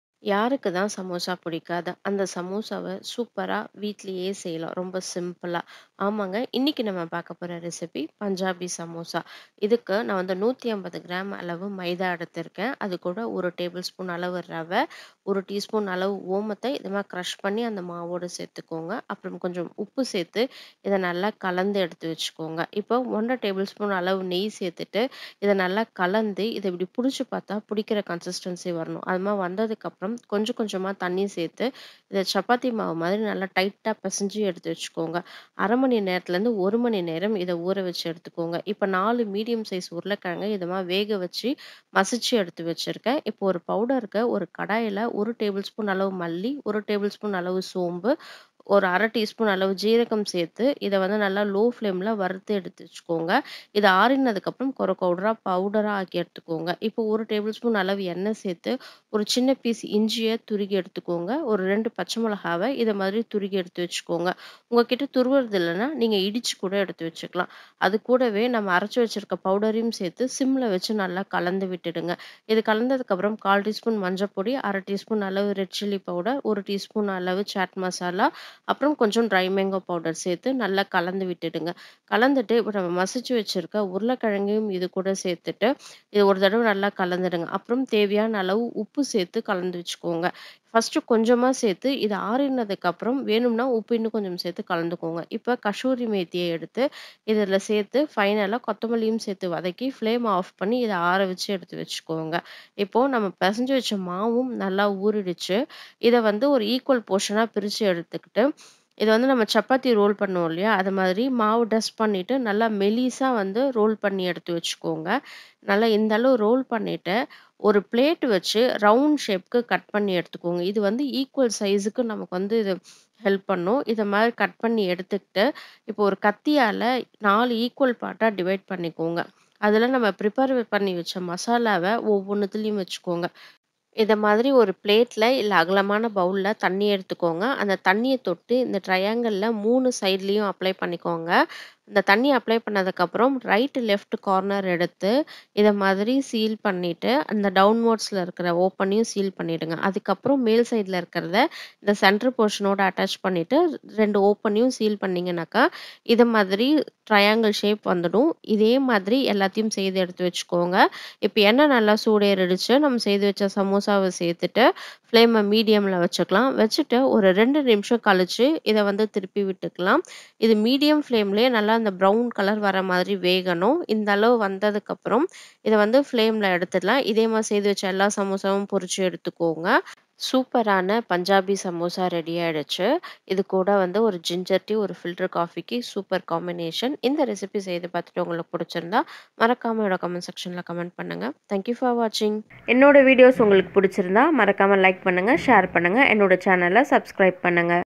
சமோசா பிரியர்களுக்கான ருசியான... பஞ்சாபி சமோசா ரெசிபி - Punjabi Samosa
Flaky and crunchy fried Samosa are one of the most popular street food snack in North Indian cuisine. No one can resist a samosa and when you serve it to your family and friends you can rest assured they will love you all the more.

Ingredients with approx measurements

Maida - 150gm
Sooji - 1 tbsp
Ajwain - 1/2 tbsp
Salt
Butter 1.5tbsp
Water
Boiled potato
Green chili
Ginger
Coriander seed - 1tbsp
Cumin seed - 1tsp
Fennel seed- 1 tbsp
Dry Mango Powder - 1tsp
Chat Masala - 1/2 tbsp
Coriander leaves
Oil

Method:

In a bowl, add 150gm of maida, 1tbsp sooji, 1 tsp ajain & salt.
Add 1.5tbsp ghee, Crumble everything well. Once the flour comes like this,
add water gradually & make a tight dough, like chapathi dough.
Rest it for 30 mins to 1 hour.
Here, i'm having boiled & mashed 4 medium size potato.
Keep it aside.
In a pan, add 1tbsp coriander seed, 1tbsp fennel seed & 1/2tsp of cumin seed.
Dry roast until it release nice aroma.Once it cools down, grind it coarsly.
In a pan, add 1 tbsp of oil. Add grated green chili, ginger & grinded powder.
Saute it in low flame.
Now add 1/4 tsp turmeric powder, 1/2tsp red chili powder, 1tsp chat masala &
dry mango power. Mix everthing well.
Add mashed potato. Saute everything well. Add salt, Kasuri methi
& coriander leaves. Mix it well, switch off the flame. Allow it to cool down.
After an hour, dough is ready. Divide it into equal parts.
Roll it like chapathi spread. Cut it asround chapathi using plate.
Cut it as 4 equal sheet. Keep the prepared stuffing in each sheet.
Take some water in plate or bowl. Apply the water in 3 sides of sheet.
Watch how I've shown it in the video. Just shape it the same way,
and if you do it like this, you'll get the perfect shape.
Now pre heated oil is ready. Pour the samosa in oil, fry in medium flame.
Until it turns nice brown in colour.
Yummy & tasty punjabi samosa is ready.
Make your evening tea time more special & tasty with this Punjabi samosa.